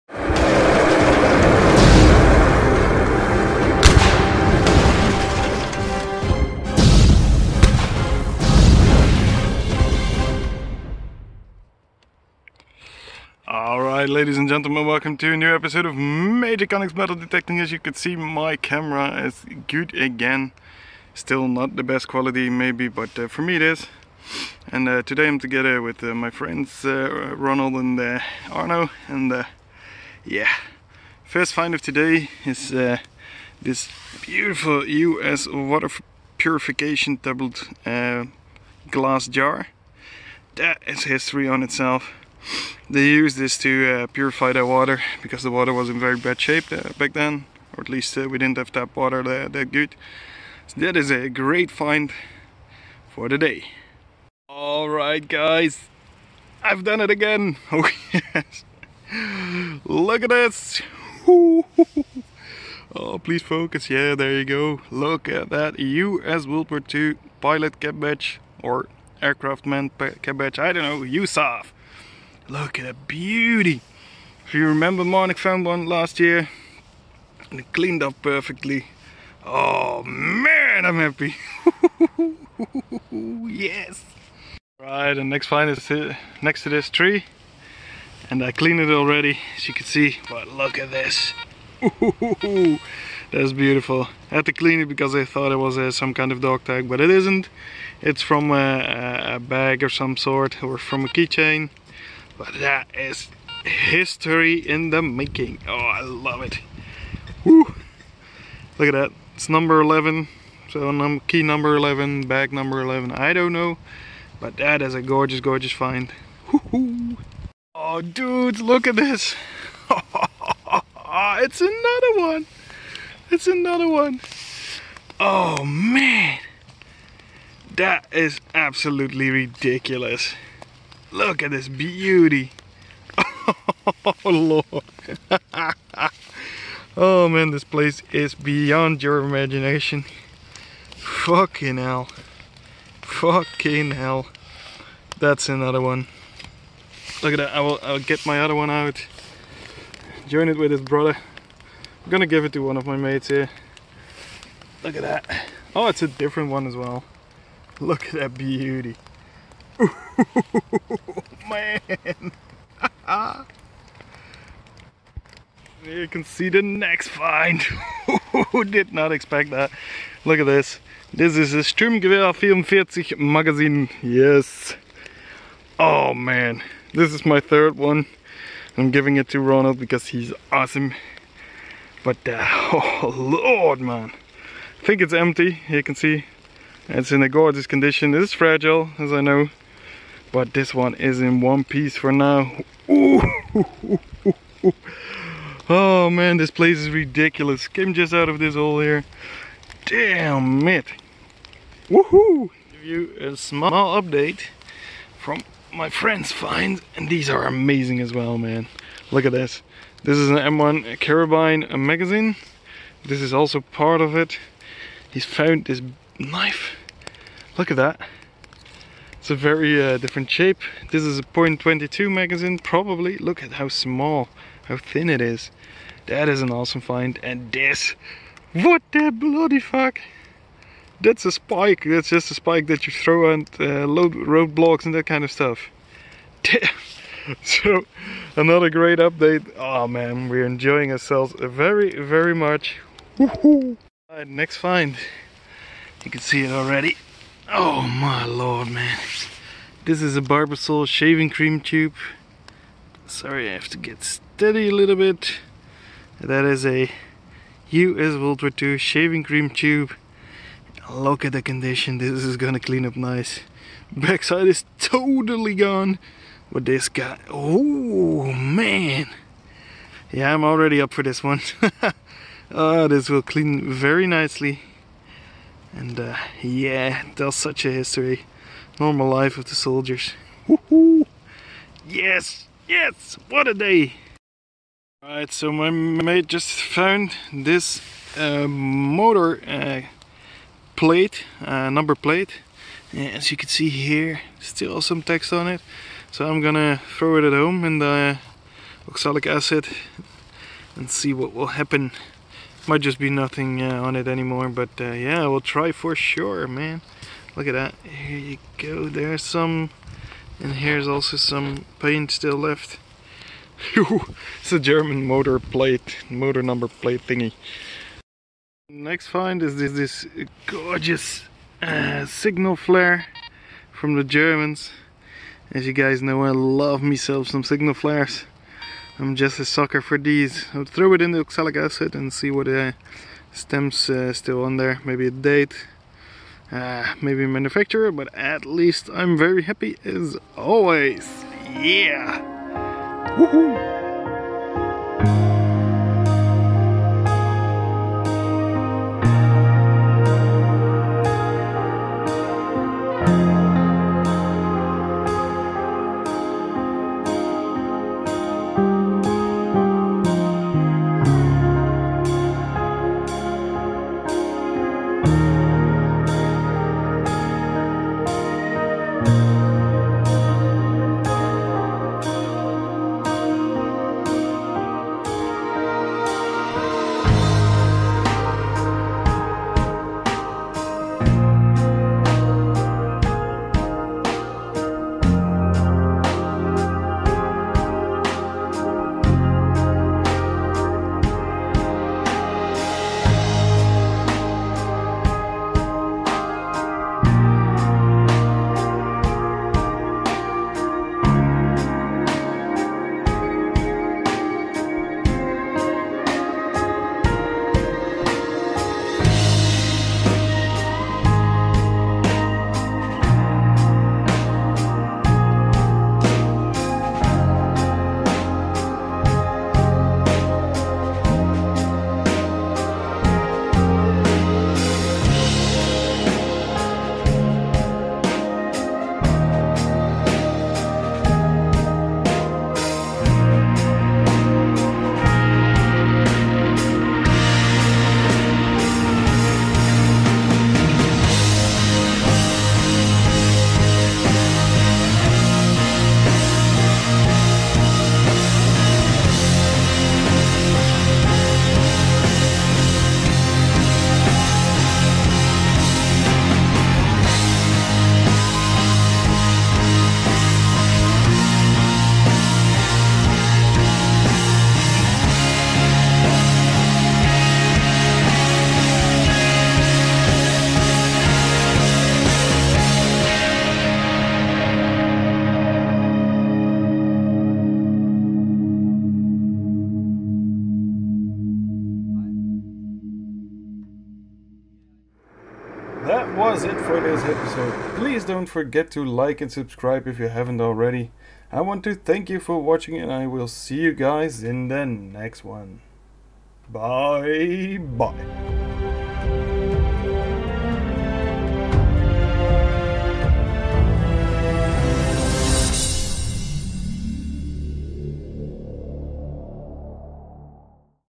WW2 Metal Detecting - US Pilot capbadges found!
Welcome to our first episode of 2019; thanks for your continues support.

This time we went to a great place where the USAAF took up positions in 1945. It was a great place to dig and even greater with my mates. Unfortunately no Marnik, but we will soon be searching together again.

Cheers & happy hunting!